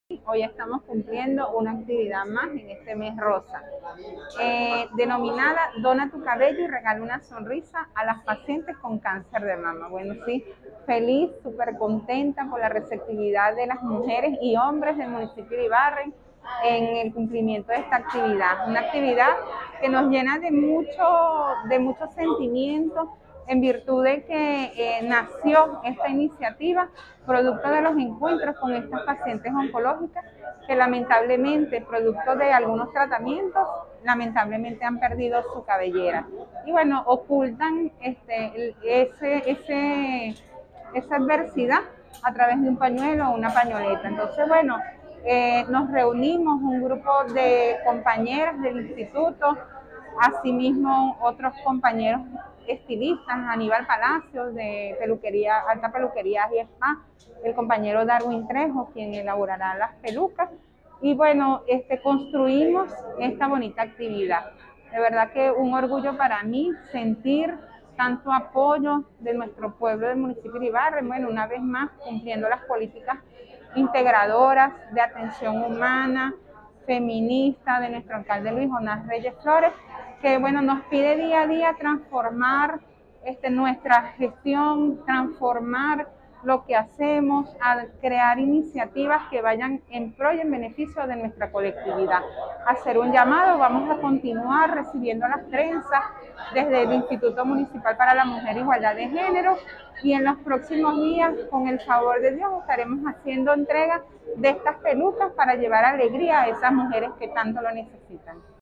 Más de 50 personas donan su cabello para pacientes con cáncer de mama
En el marco de la celebración del día de la prevención de cáncer de mama, una alta sensibilidad y amor al prójimo demostraron 50 personas que donaron su cabello a pacientes que lo perdieron durante su tratamiento contra el cáncer de mamas, la presidenta Alba López, del Instituto municipal para la mujer e igualdad de género, realizó una jornada denominada «Dona tu cabello y regala una sonrisa a las pacientes con cáncer de mama».

Penélope Daza, donante de 17 con su empatía por esta causa dijo: «Me siento feliz y emocionada por saber que este cabello va a ser donado a pacientes con cáncer».